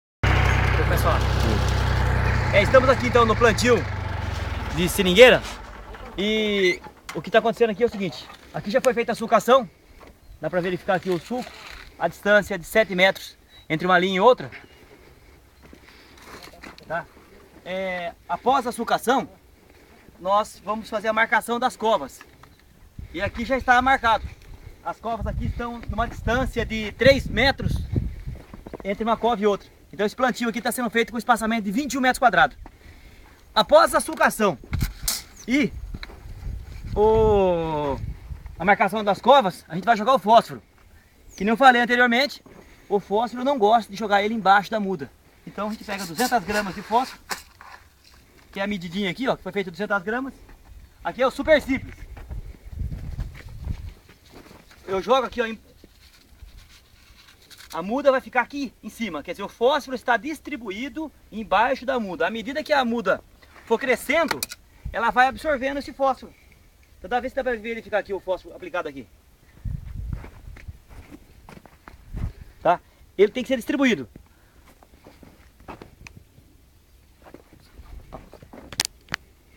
Plantando seringueira. Aplicação de fonte de fósforo (Supersimples) no sulco
Neste vídeo o engenheiro agronomo Marcos Murbach mostra a técnica recomendada para aplicação da fonte de fosforo Supersimples no sulco durante o plantio.